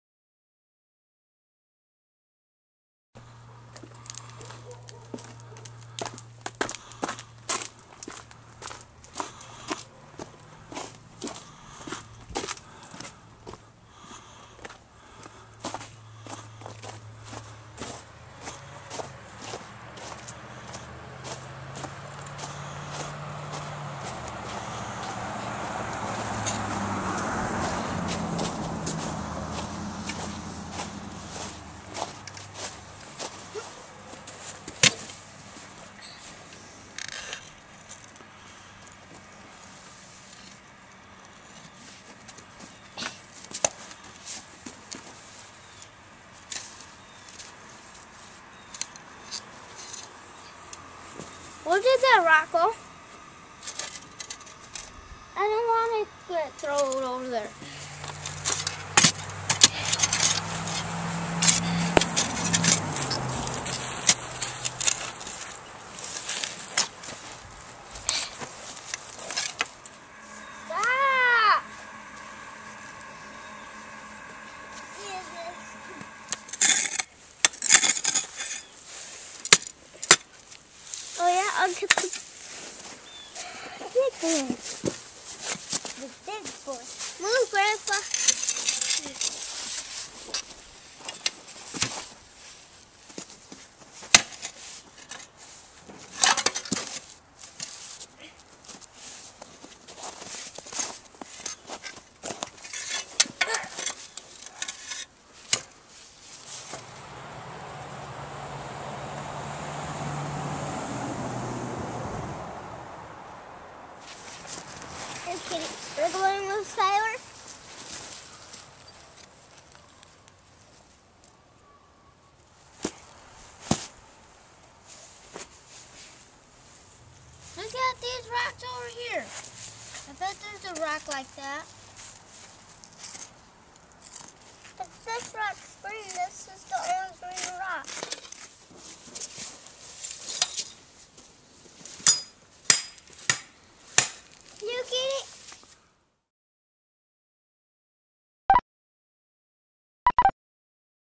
Digging a hole to China
So what if the ground is frozen?  It's no time to stop digging for China.  Skylar and Isiah put away their sleds and get out the shovels.